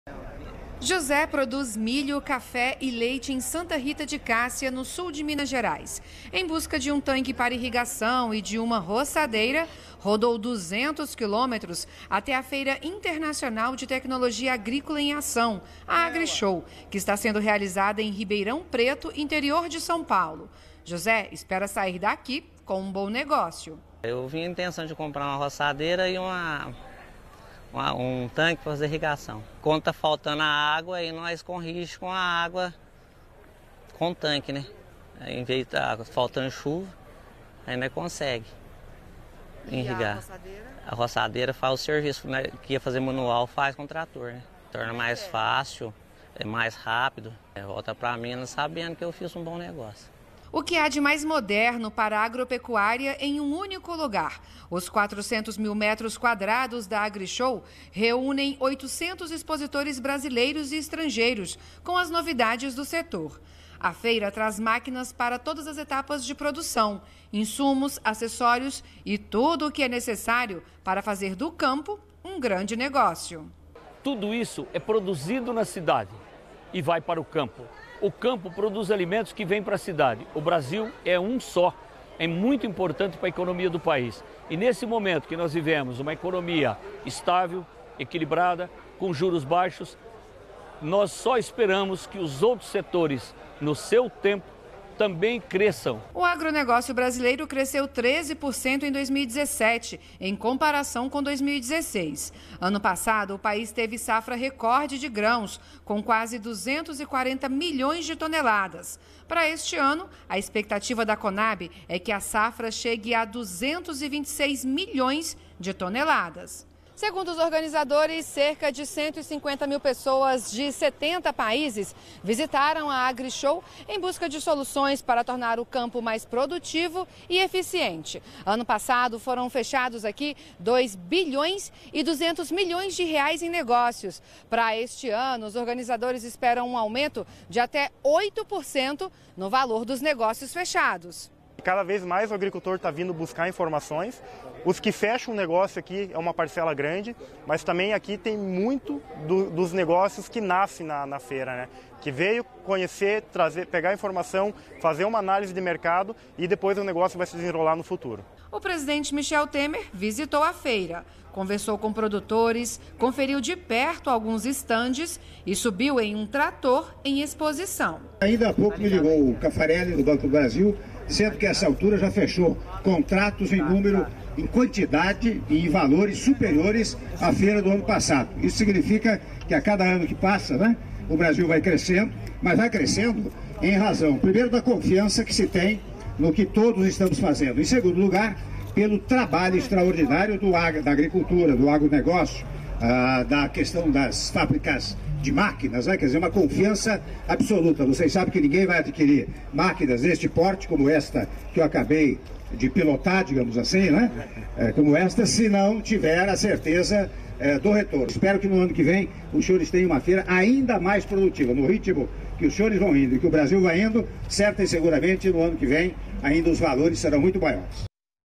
Presidente Michel Temer visita feira Agrishow
REPÓRTER NBR - 03.05.18: O presidente Michel Temer visitou na manhã desta quinta-feira uma das mais importantes vitrines tecnológicas do país, a Agrishow, no município paulista de Ribeirão Preto. O agronegócio brasileiro cresceu 13% em 2017, em comparação com 2016. Um resultado que se confirmou nos bons  negócios fechados na feira deste ano.